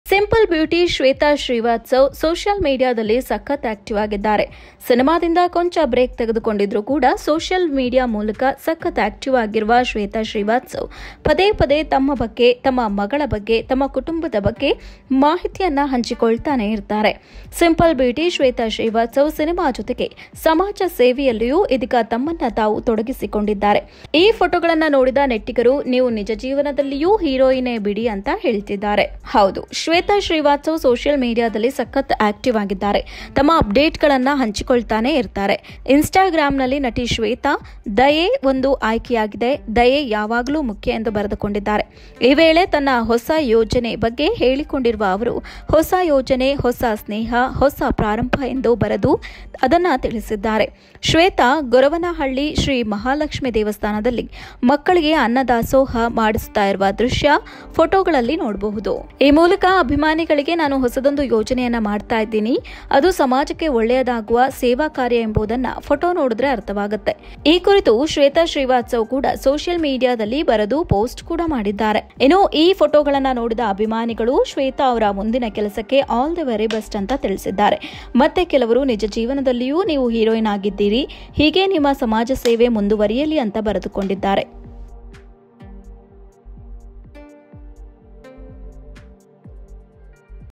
Shwetha Srivatsav | ಶ್ವೇತಾ ಶ್ರೀವಾತ್ಸವ್ ಕೆಲಸಕ್ಕೆ ಅಭಿಮಾನಿಗಳಿಂದ ಮೆಚ್ಚುಗೆ
kindness always matters shwetha srivatsav give social message

ಸಿಂಪಲ್ ಬ್ಯೂಟಿ ಶ್ವೇತಾ ಶ್ರೀವಾತ್ಸವ್ ಸಿನಿಮಾ ಜೊತೆಗೆ ಸಮಾಜಸೇವೆಯಲ್ಲಿಯೂ ತಮ್ಮನ್ನು ತಾವು ತೊಡಗಿಸಿಕೊಂಡಿದ್ದು, ಅವರ ಫೋಟೋಗಳನ್ನು ನೋಡಿ ನೆಟ್ಟಿಗರು ನೀವು ನಿಜ ಜೀವದಲ್ಲಿಯೂ ಹೀರೋಯಿನ್ ಆಗಿದ್ದೀರಾ ಎಂದು ಪ್ರಶಂಸುತ್ತಿದ್ದಾರೆ.